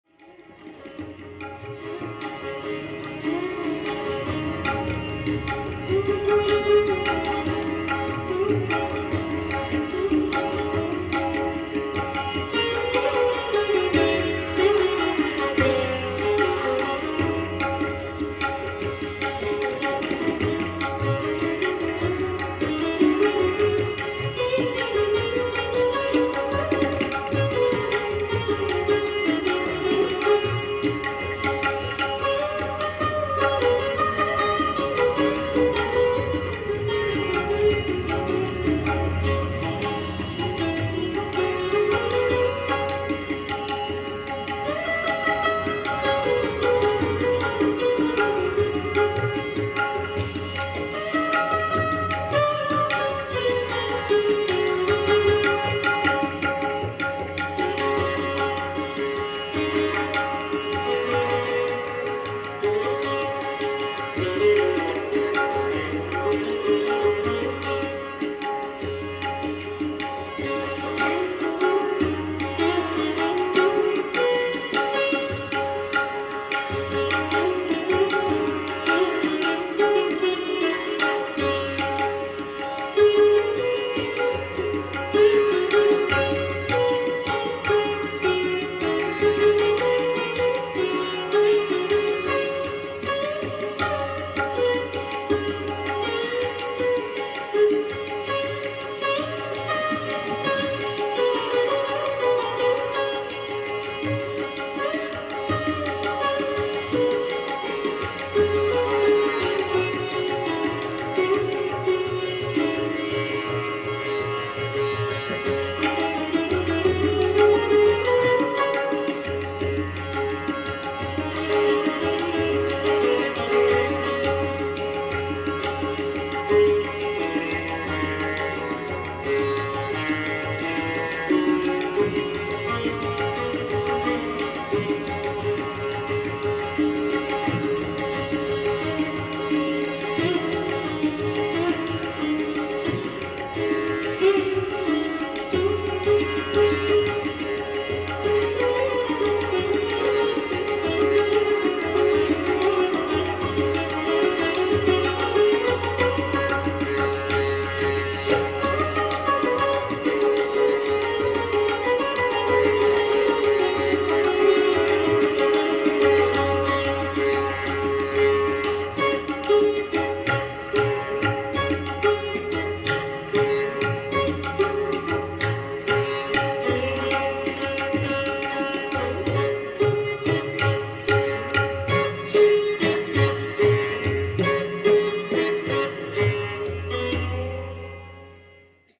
Kanala & Sadanand April 12 2023
Performance at Aspiration-Ground meditation gardens in Jamaica Hills, Queens, New York.